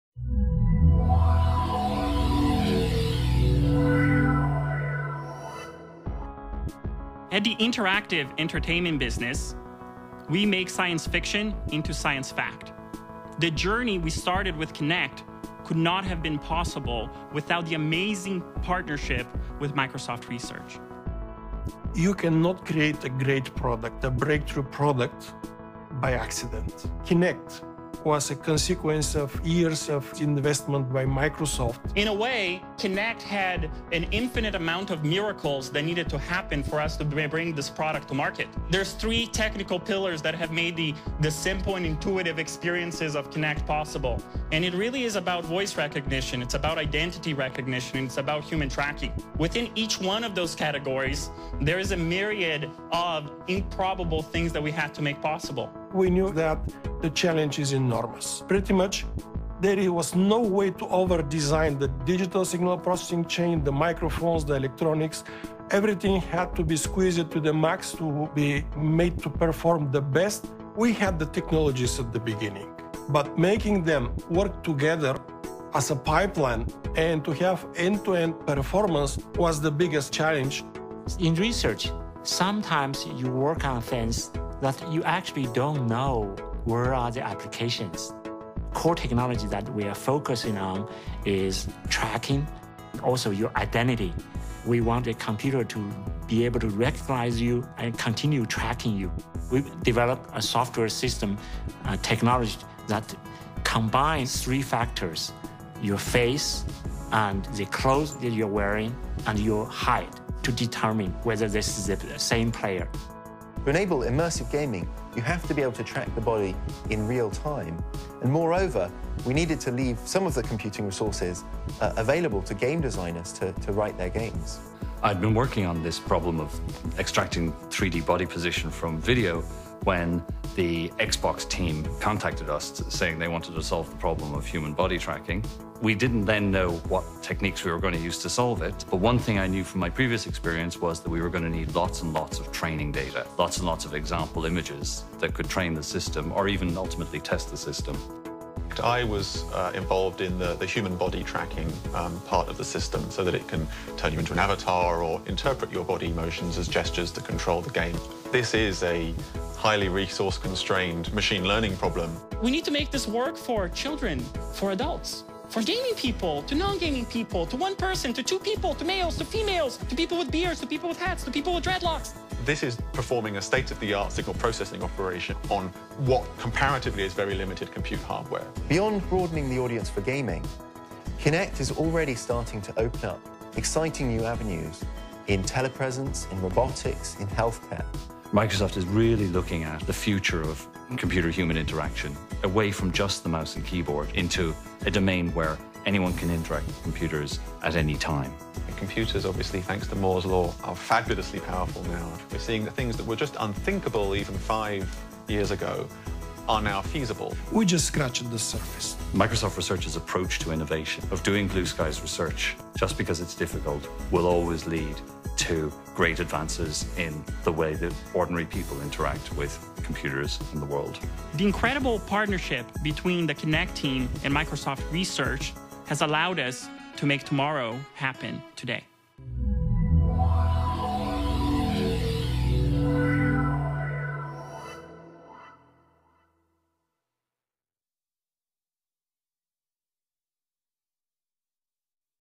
Researchers Highlight Collaborations with Kinect for Xbox 360
Microsoft researchers around the globe highlight research contributions to Kinect for Xbox 360 and collaborations with the team that developed the product.